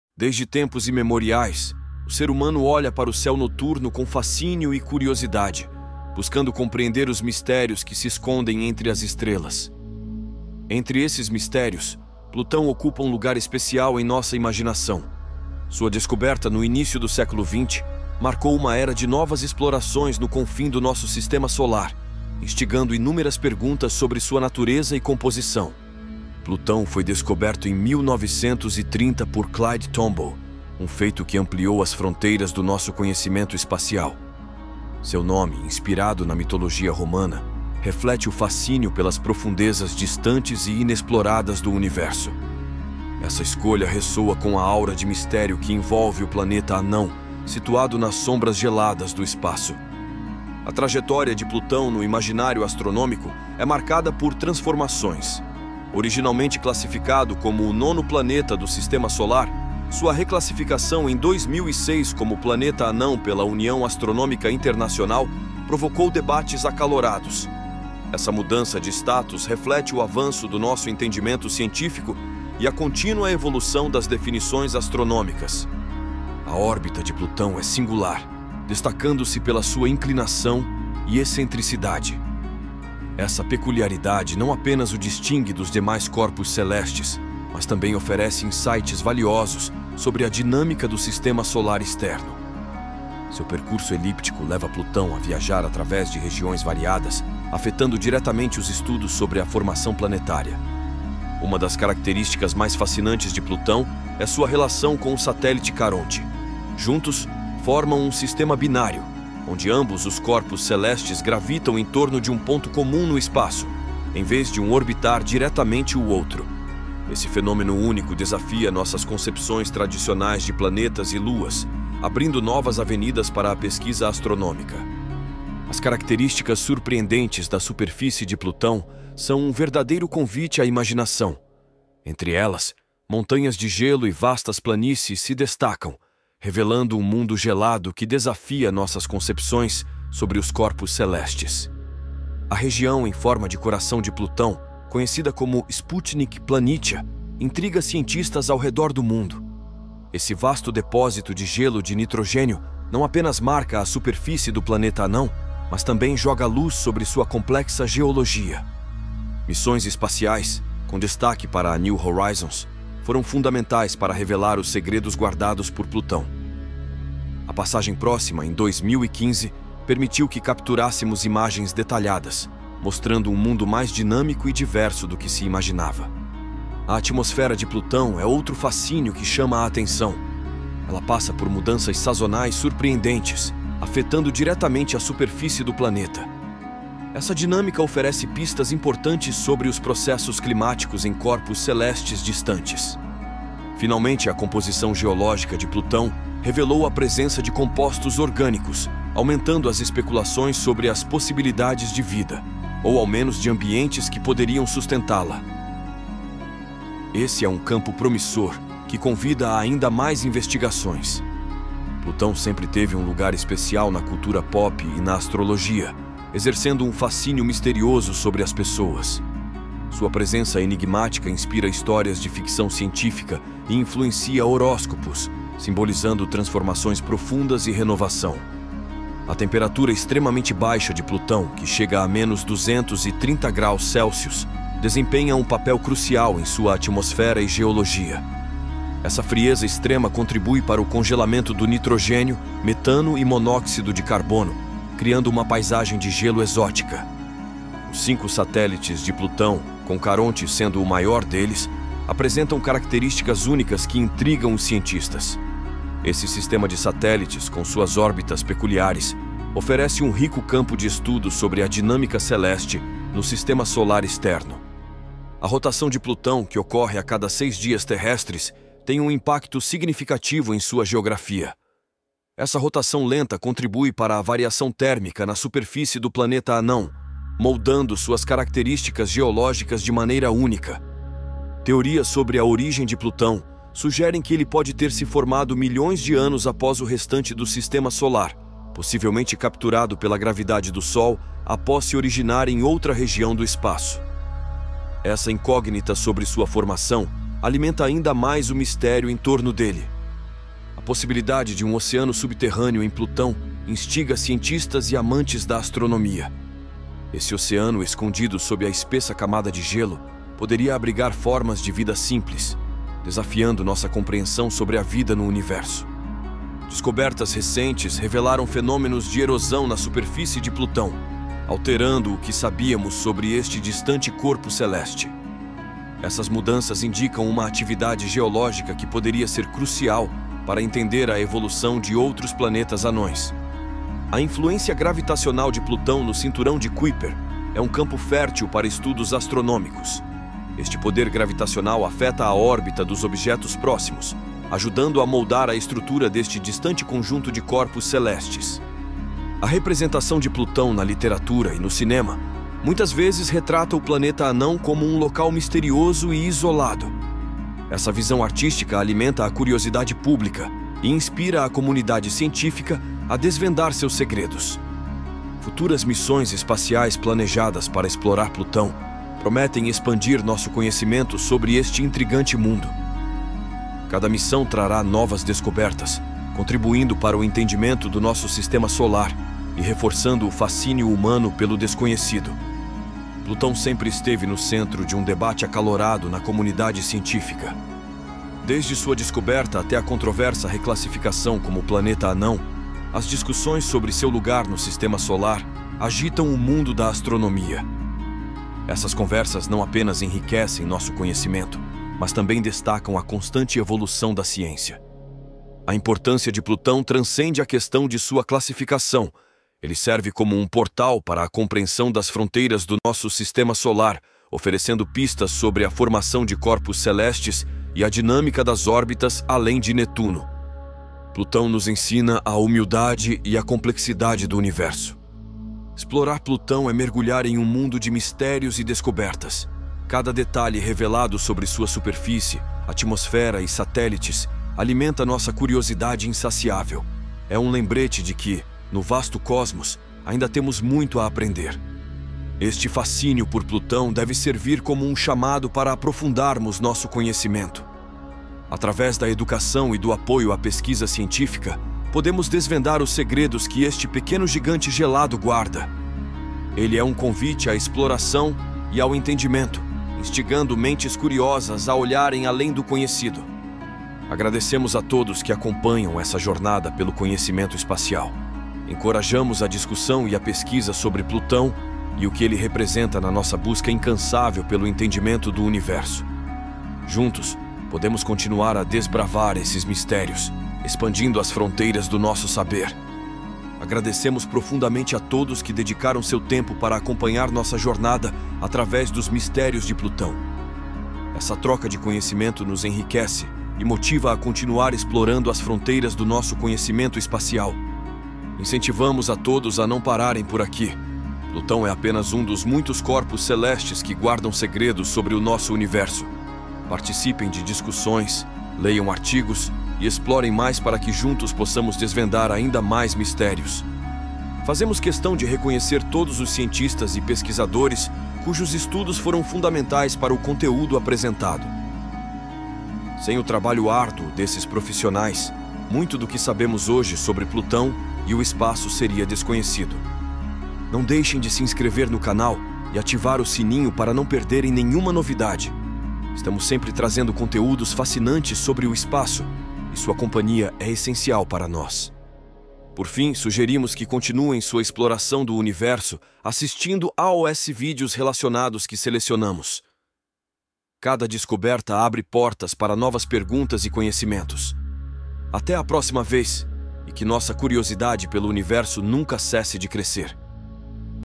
O Enigma do Sistema Solar
Neste vídeo, embarque conosco numa jornada ao confim do nosso sistema solar para desvendar os mistérios de Plutão. Desde sua descoberta no início do século XX, Plutão tem fascinado cientistas e astrônomos, gerando curiosidade sobre suas características únicas e sua composição. Explore conosco a história, as descobertas e as recentes missões que nos ajudam a entender melhor este corpo celeste enigmático.